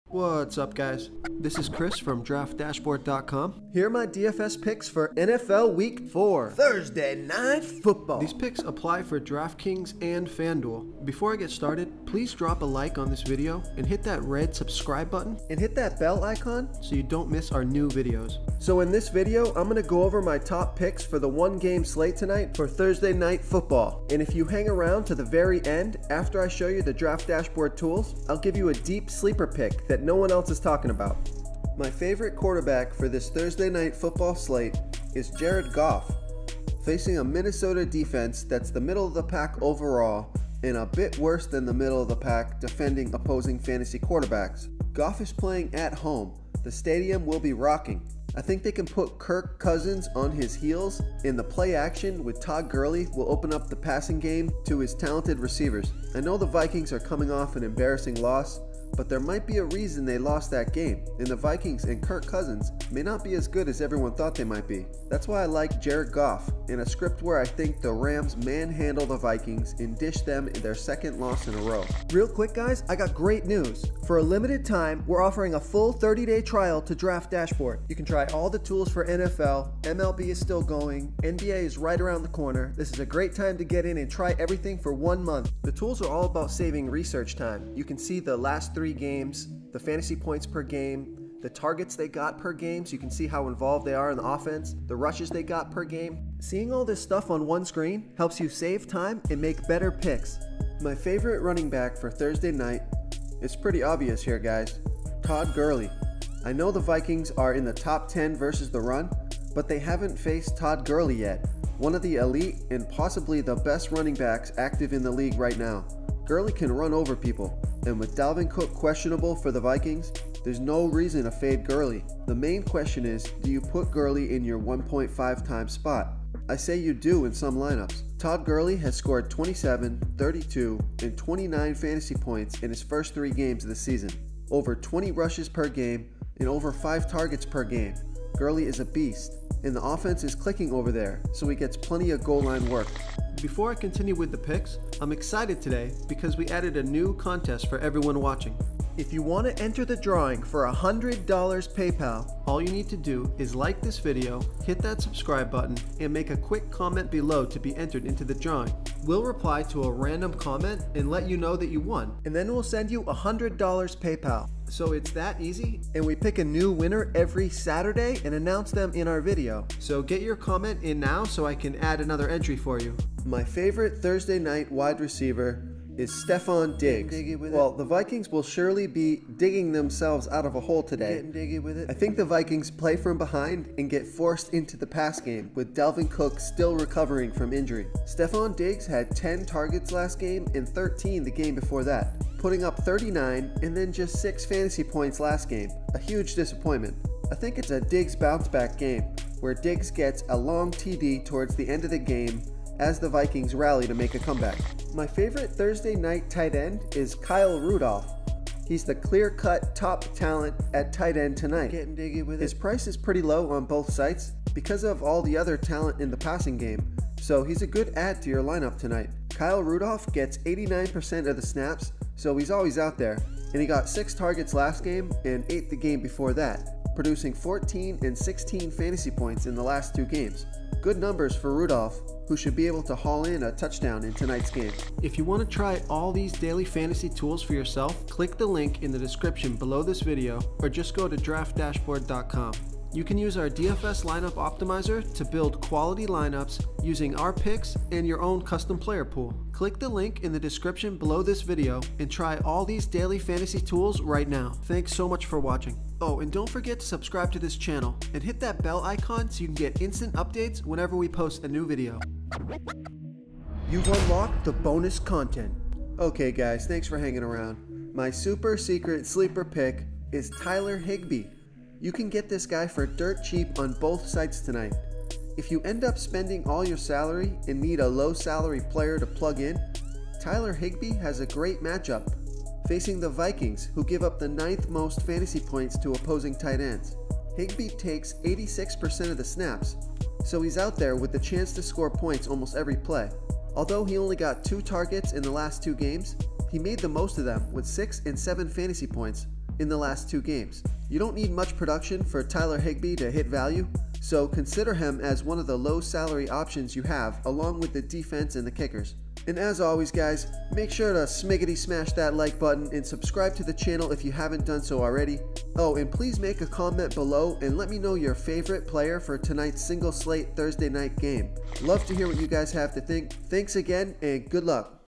DraftKings Picks Week 4 NFL Thursday Night Football
DraftKings Picks Week 4 NFL Thursday Night Football Single Game Slate! Try our DFS Tools and improve your DraftKings and FanDuel Picks for Daily Fantasy Football.